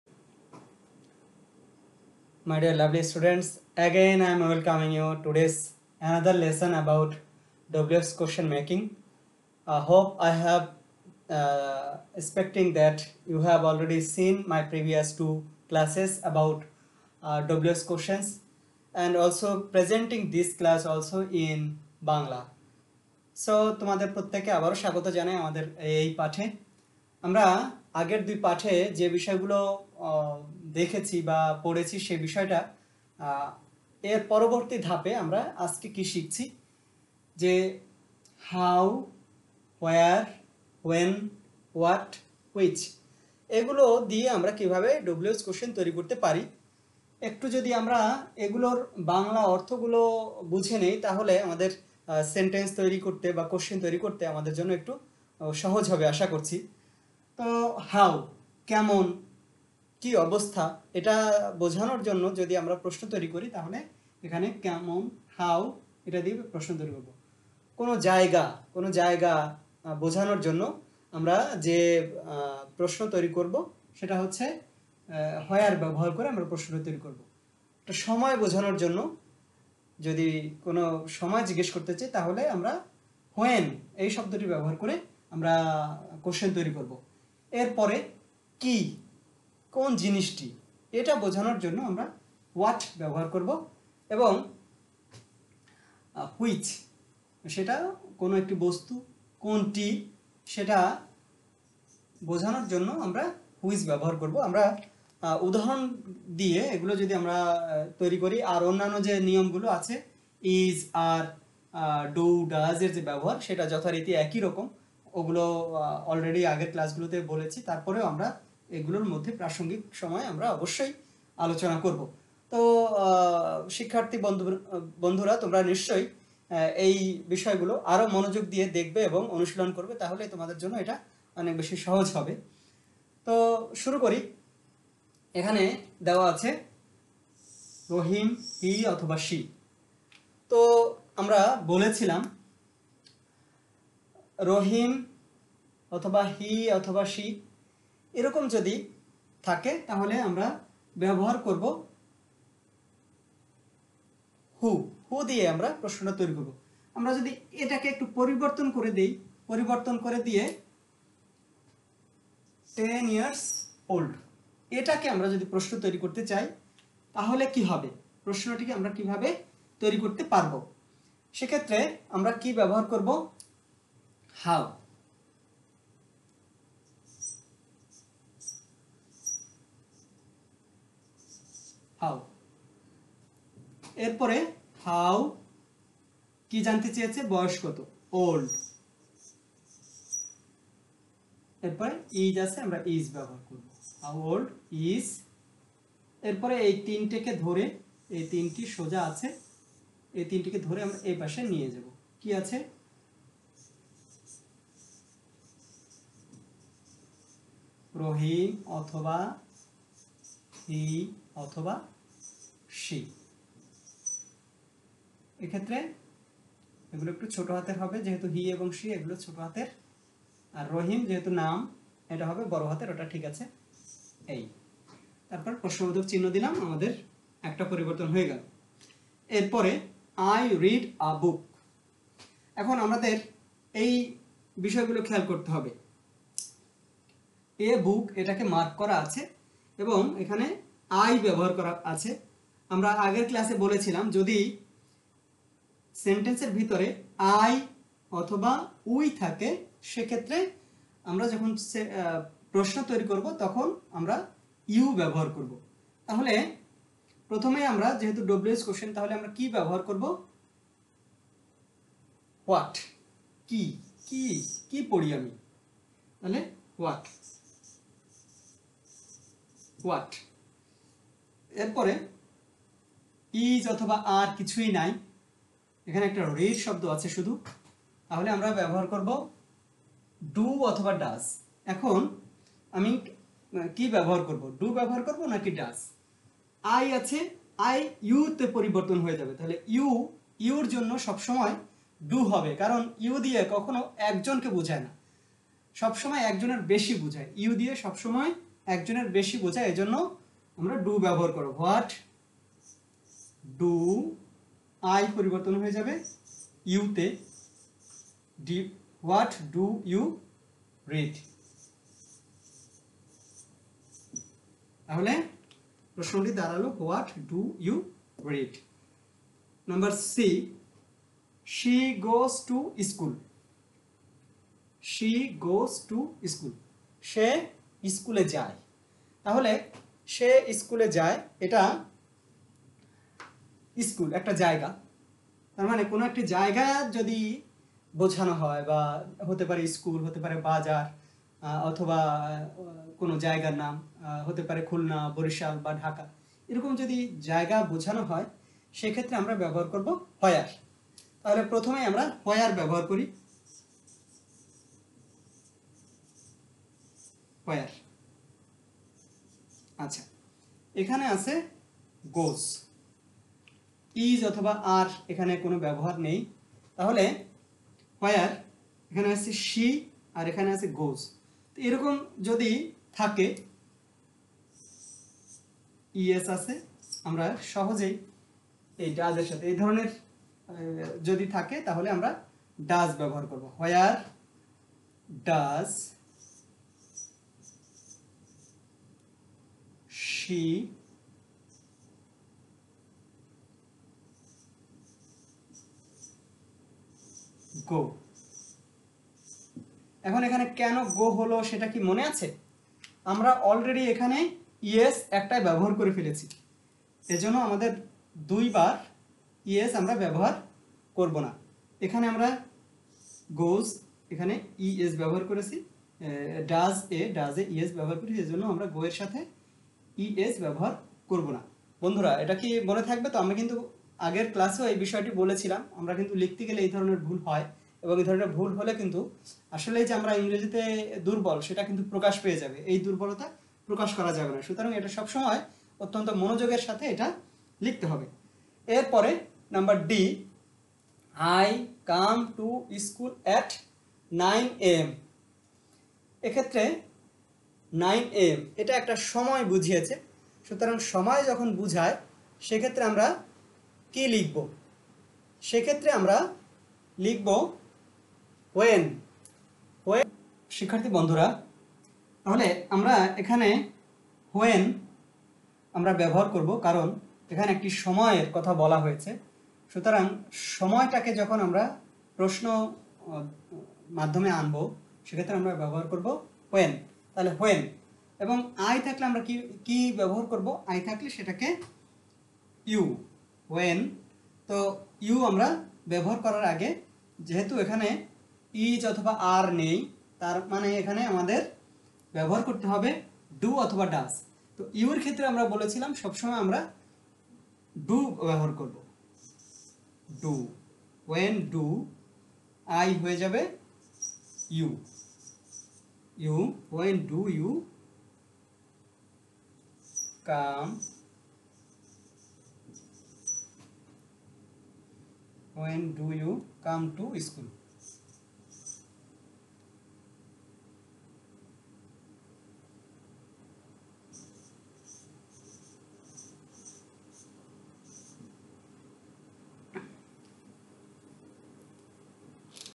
WH Question Making lesson 3
শিক্ষার্থীরা  how,what,when and where ব্যবহার করে W H Question making সম্পর্কে আরো বিশদভাবে জানতে পারব। আরো ভালো বোঝার জন্য আগের পাঠের তুলনায় একটু বিশদভাবে আলোচনা করা হয়েছ।

Students will learn more about W H Question making using how,what,when and where. I little more detail is described here for better understanding.

আগের ক্লাশের ভিডিও লঙ্ক-